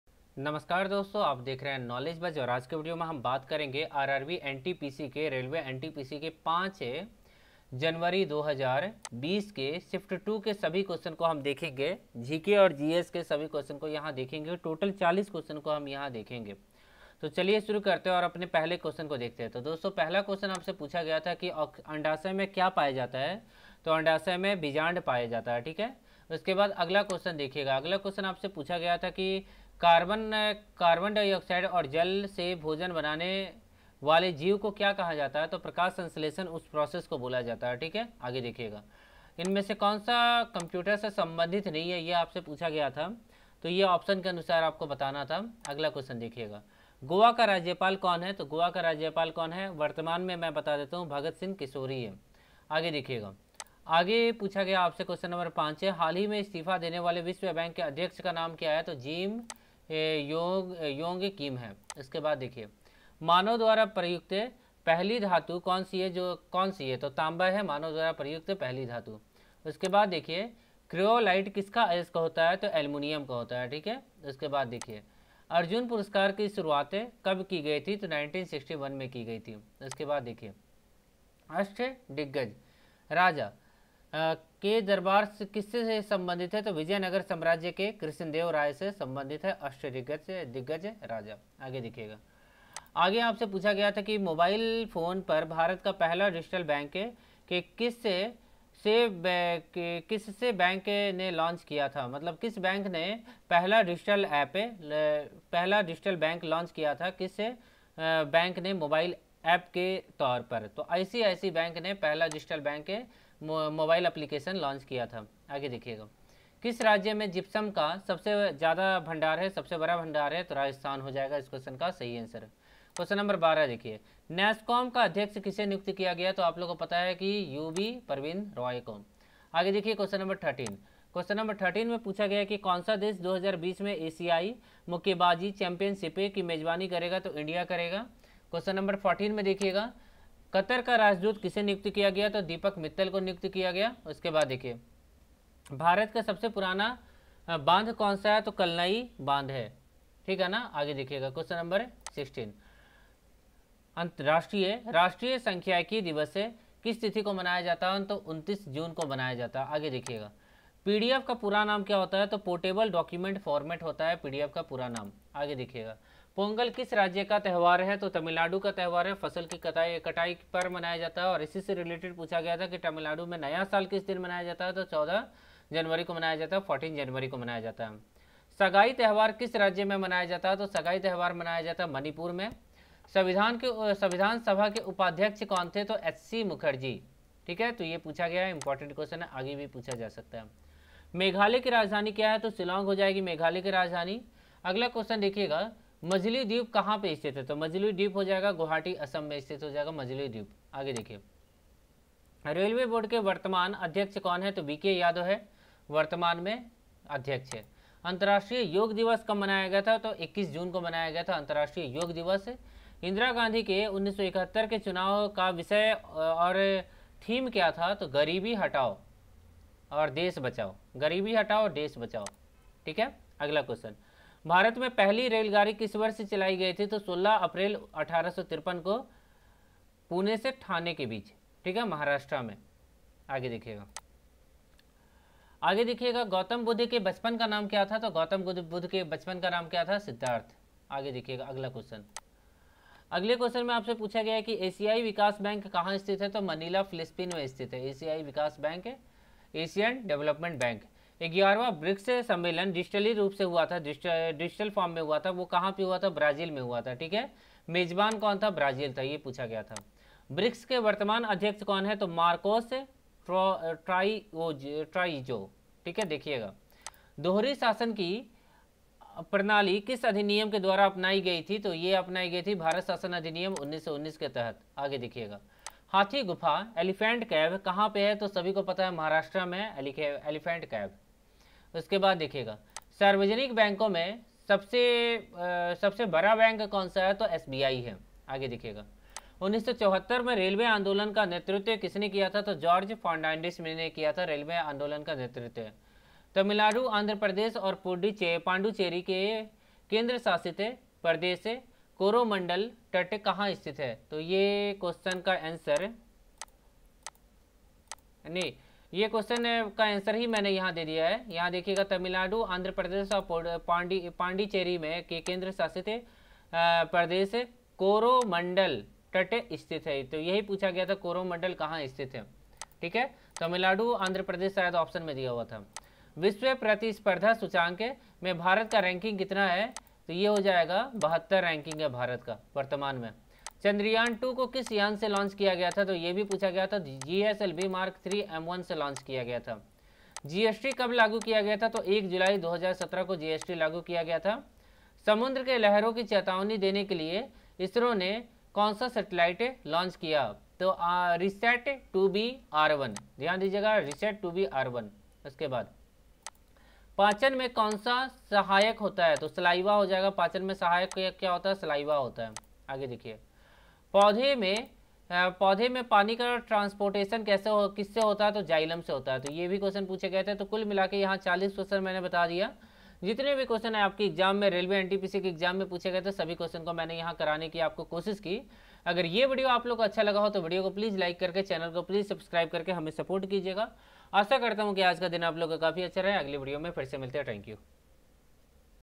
Railway RRB NTPC Exam Analysis : 5 January Second Shift | All 40 asked in GK | GS
Previous GK and GS all question Railway ntpc question
Based on NCERT book up 10 level